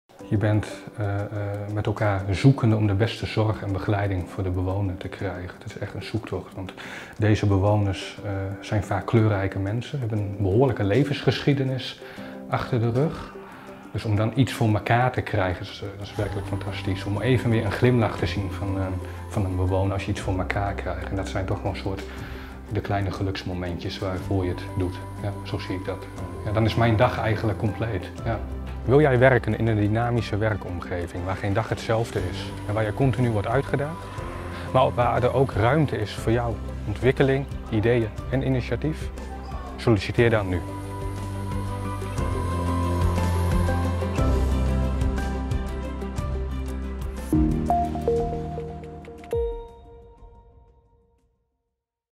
Werken bij Atlant - Marken Haven: Gerontopsychiatrie
‘Hier wonen kleurrijke mensen met een behoorlijke levensgeschiedenis. En om dan een glimlach van een bewoner te zien. Dat is fantastisch. Dan is mijn dag compleet.’ Lorenzo werkt op Atlantlocatie Marken Haven met mensen met een gerontopsychiatrische zorgvraag.